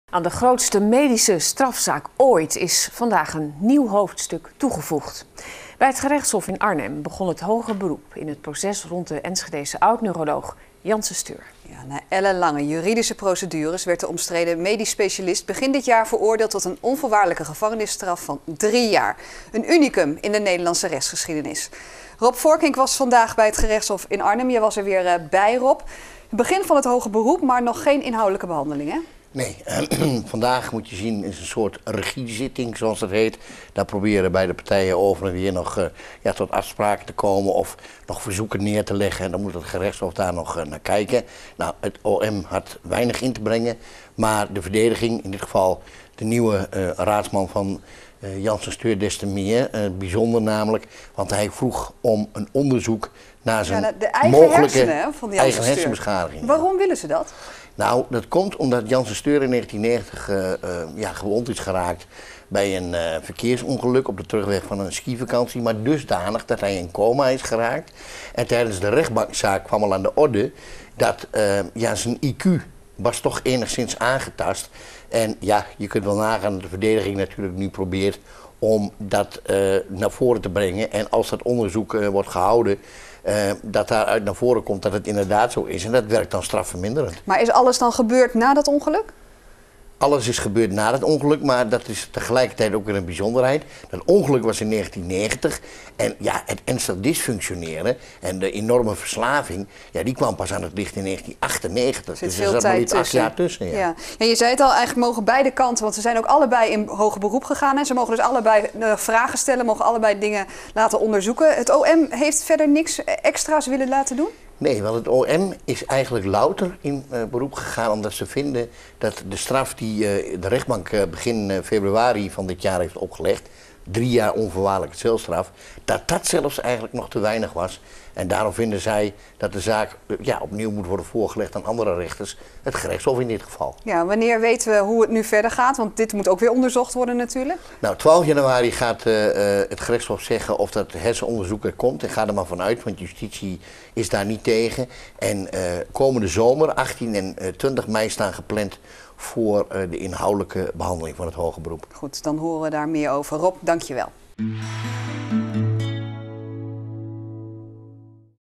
"Ex-neuroloog Jansen Steur moet opnieuw psychisch onderzocht worden"
Ex-neuroloog Ernst Jansen Steur moet opnieuw psychisch onderzocht worden. Dat wil zowel het Openbaar Ministerie als Jansen Steur zelf. Dat bleek vandaag tijdens een regiezitting van het hoger beroep tegen de voormalig arts. In januari wordt besloten of het nieuwe psychische onderzoek er ook echt komt.